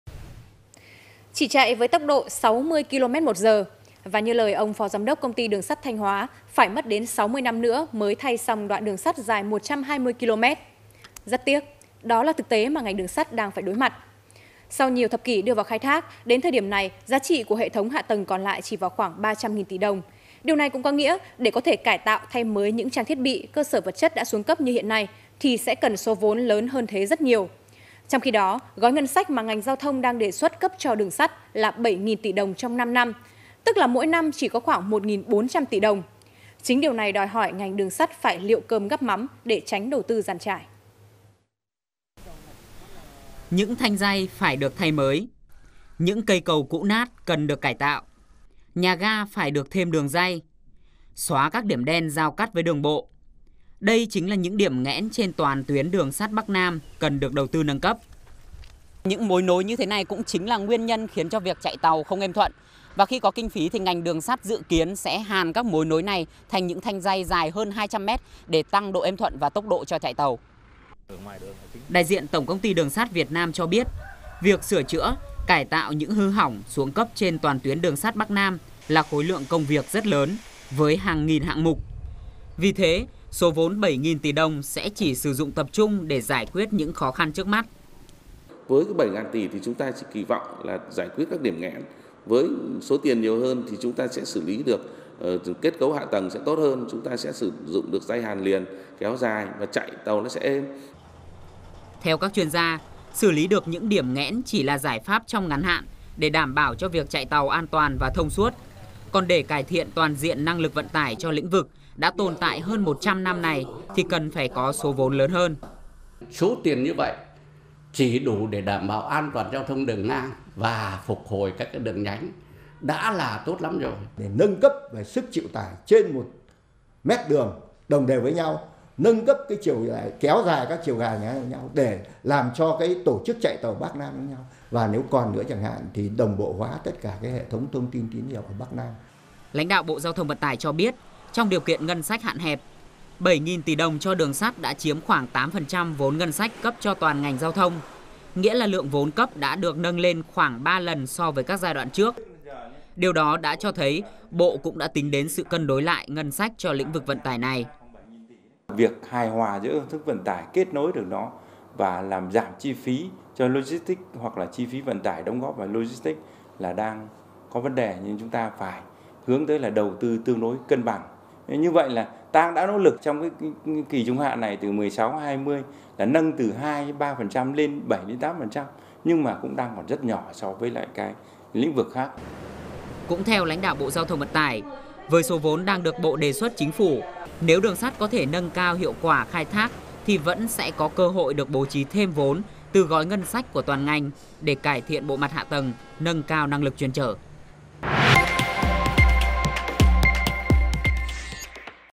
Ngành Đường Sắt Gỡ Nút Thắt Hạ Tầng - Tin Tức VTV24
Với 7000 tỷ đồng trong 5 năm ngành đường sắt nước ta chỉ có thể làm được những gì?